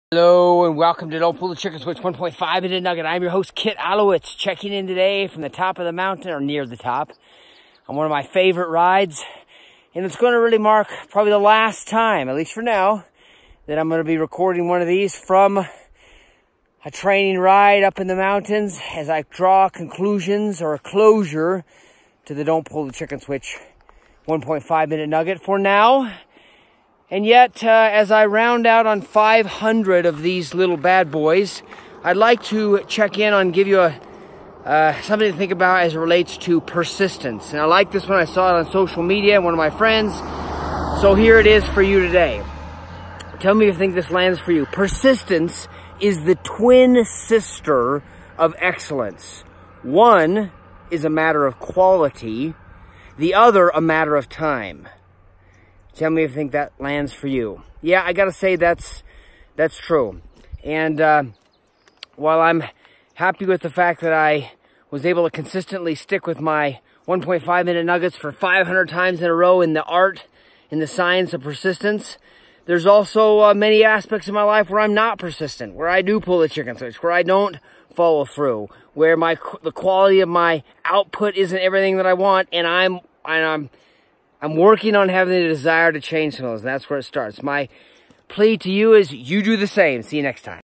1.5 minute nugget. Topic: ‘Persistence is the twin sister of excellence.....One is a matter of .....
1.5 minute nugget. Topic: ‘Persistence is the twin sister of excellence. One is a matter of quality; the other, a matter of time!’ Habeeb Akande